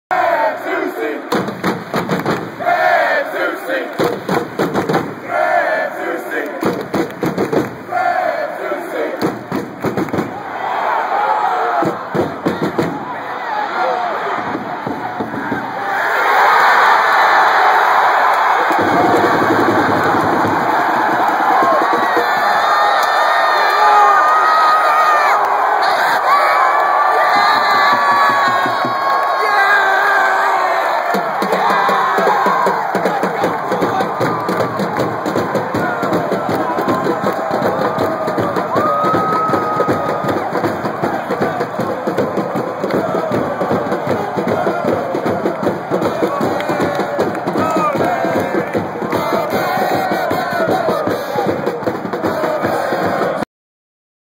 Jozy Altidore scores for USMNT vs. Jamaica, American Outlaws fans go wild
The moments right before and right after Jozy Altidore scored against Jamaica to give the USA Soccer Team a 2-0 lead. I tried to capture the enthusiasm in the American Outlaws fan section